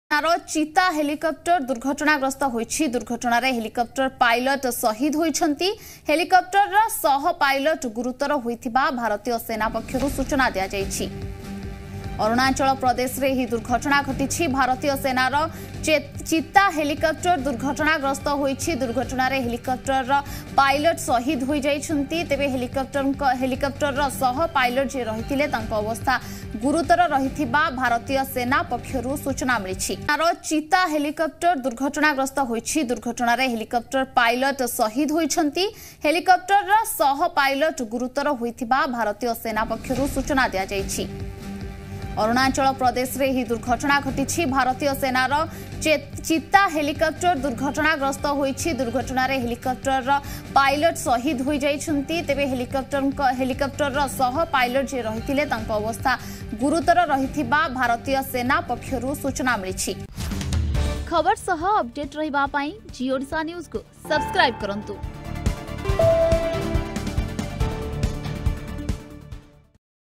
BREAKING: Indian Army Cheetah Helicopter Crashes Near Tawang | ଭାରତୀୟ ସେନାର ଚିତା ହେଲିକାପ୍ଟର ଦୁର୍ଘଟଣା
BREAKING: Indian Army Cheetah Helicopter Crashes Near Tawang | ଭାରତୀୟ ସେନାର ଚିତା ହେଲିକାପ୍ଟର ଦୁର୍ଘଟଣାଗ୍ରସ୍ତ, ପାଇଲଟ ମୃତ

About Channel:-